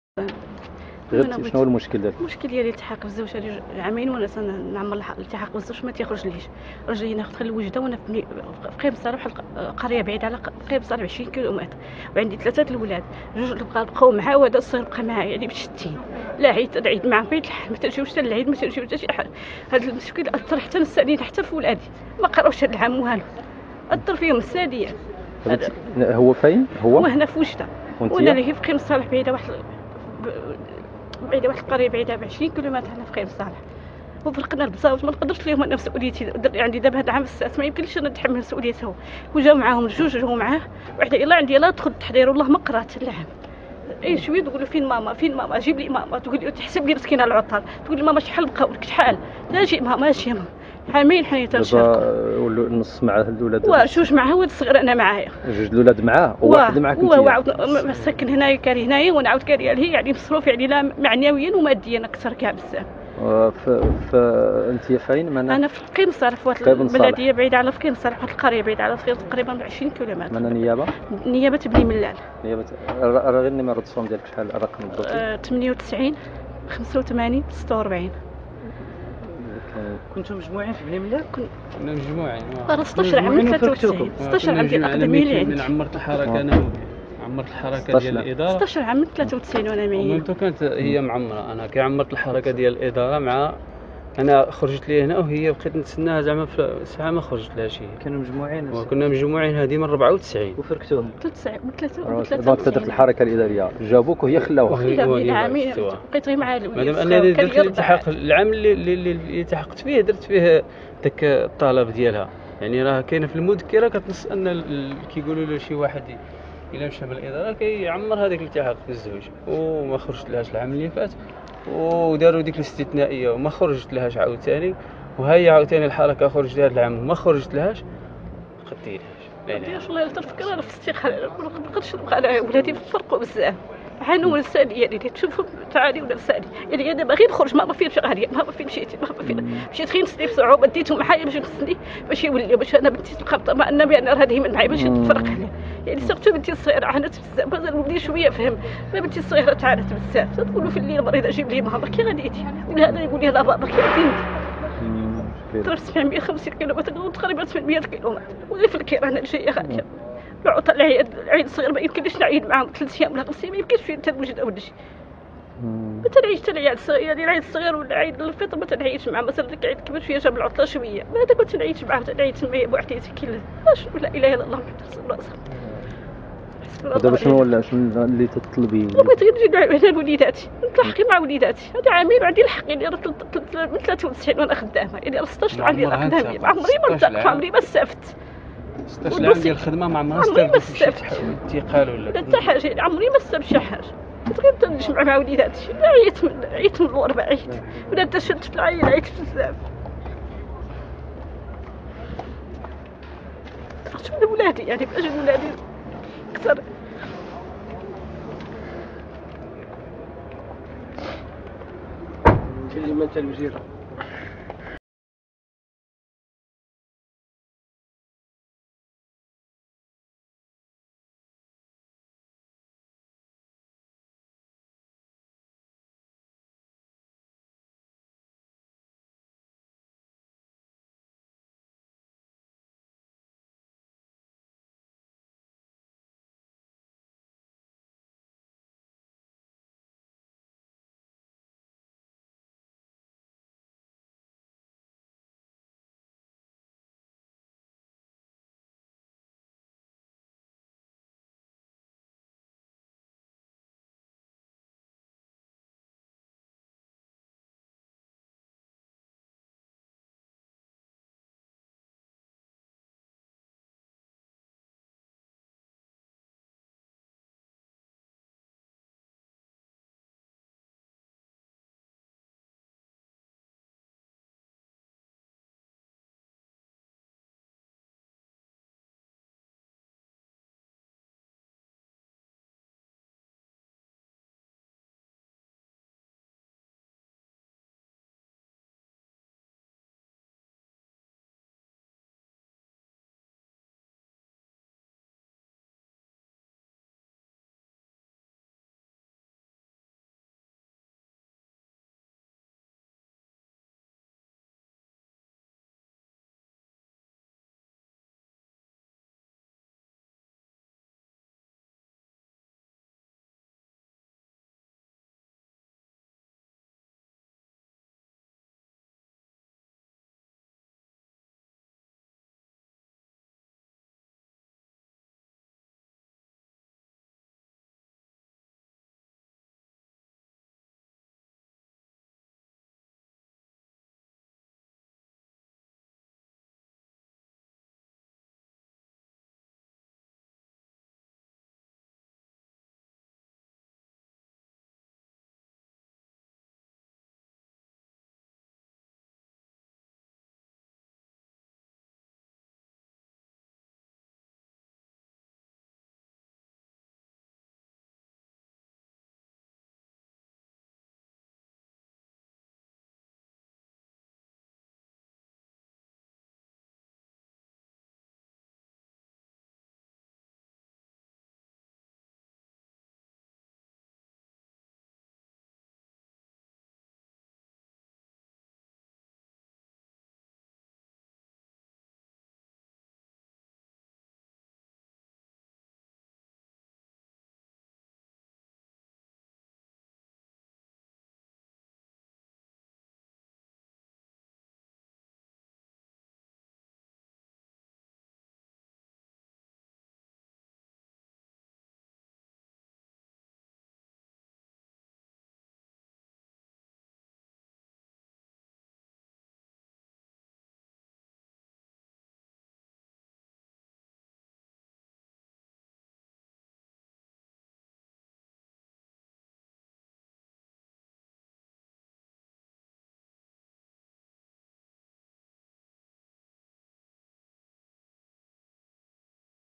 04/08/2009 - oc-proff-fkihbensalah-men
une professeur de fkih bensalah demande  au ministre de l ' education national  de rejoindre son mari a oujda